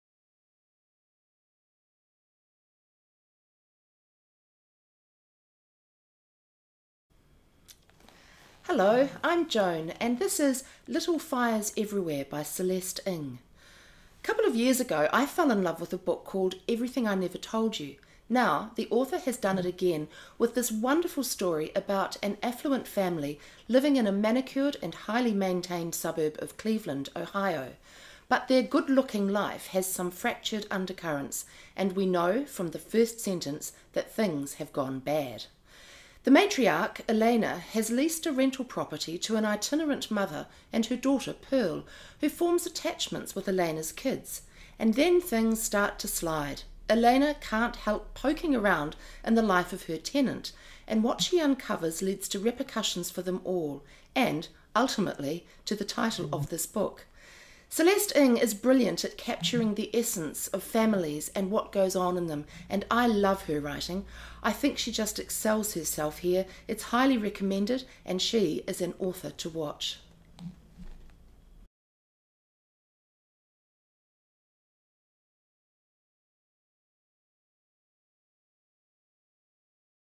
Joan Mackenzie reviews Little Fires Everywhere by Celeste Ng
Hello, I’m Joan and this is Little Fires Everywhere by Celeste Ng. A couple of years ago I fell in love with a book called Everything I Never Told You. Now the author has done it again with this wonderful story about an affluent family living in a manicured and highly maintained suburb of Cleveland, Ohio – but their good looking life has some fractured undercurrents, and we know from the first sentence that things have gone bad. The matriarch, Elena, has leased a rental property to an itinerant mother and her daughter, Pearl, who forms attachments with Elena’s kids – and then things start to slide. Elena can’t help poking round in the life of her tenant, and what she uncovers leads to repercussions for them all – and, ultimately, to this book’s title. Celeste Ng is brilliant at capturing the essence of a family, and here she just excels herself. Highly recommended, and an author to watch.